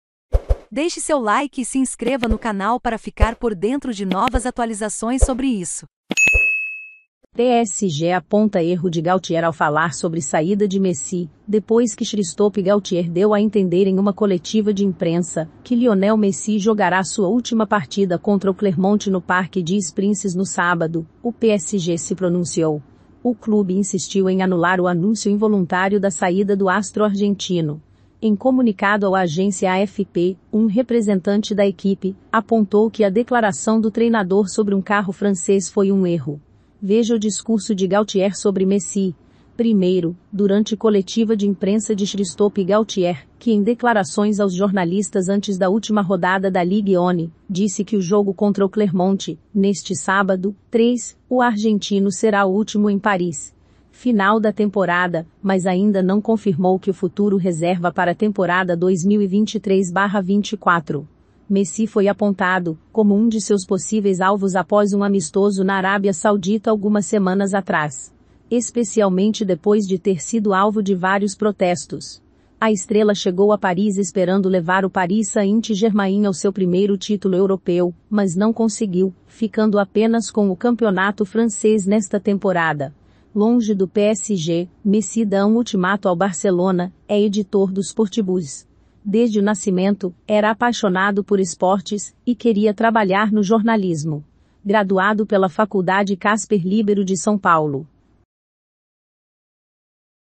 PSG APONTA ERRO DE GALTIER AO FALAR SOBRE SAÍDA DE MESSI
Confira o discurso de Galtier sobre Messi! Como esperado. Lionel Messi não vai ficar no PSG. Longe do PSG. Messi dá um ultimato ao Barcelona. ​​é editor do SportBuzz.